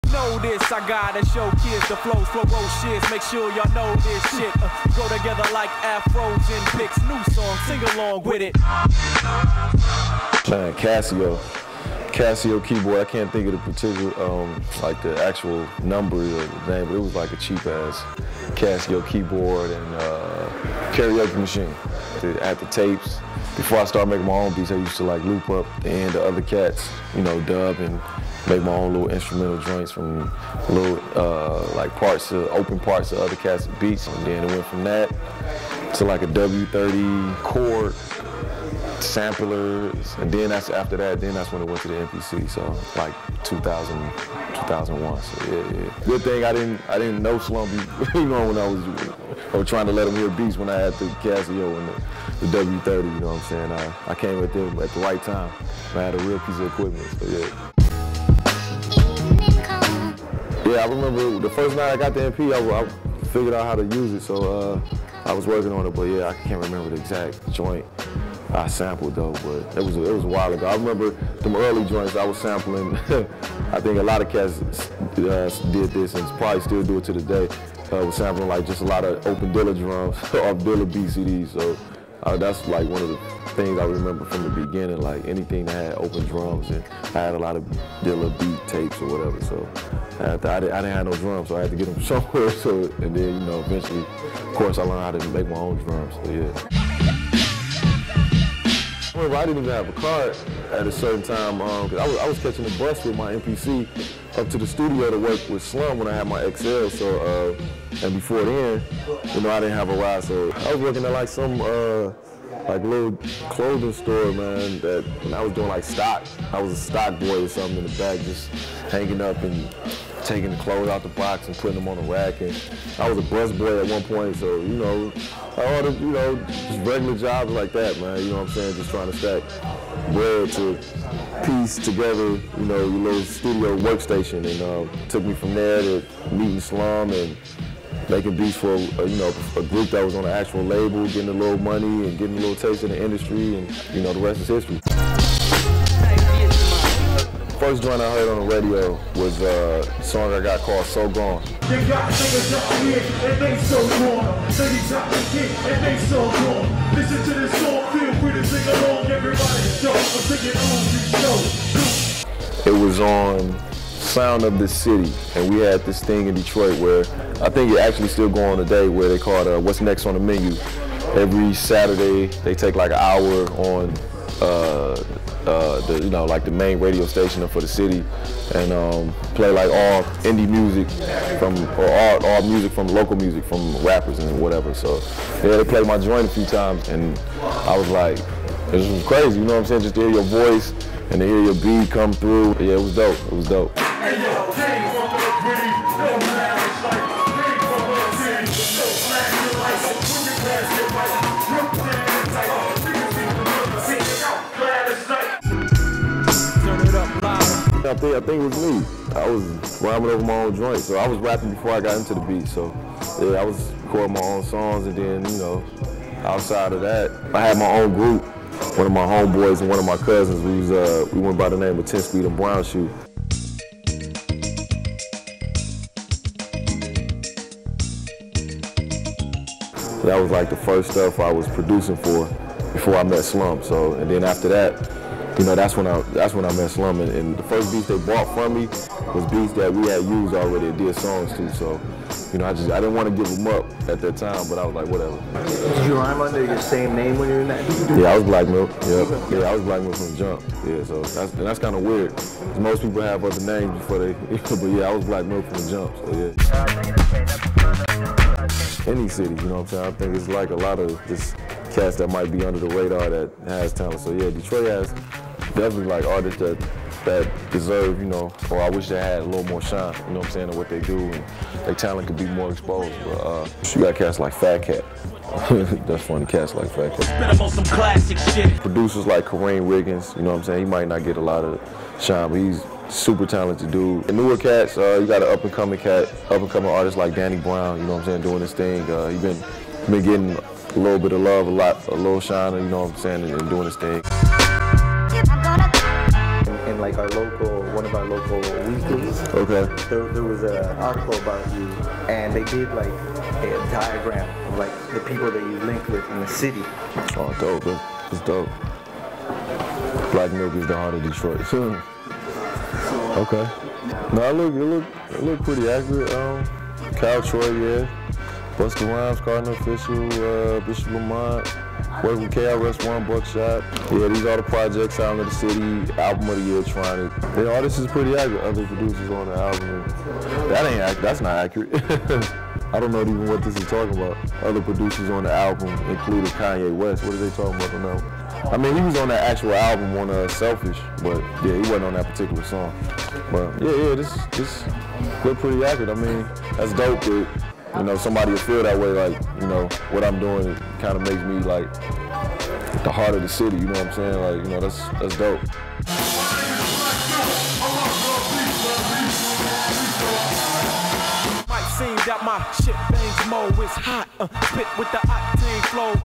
Black Milk x Pound Magazine
Black Milk tells Pound Magazine about the first beat he ever made, the first time he heard his song on the radio, his first job, and more.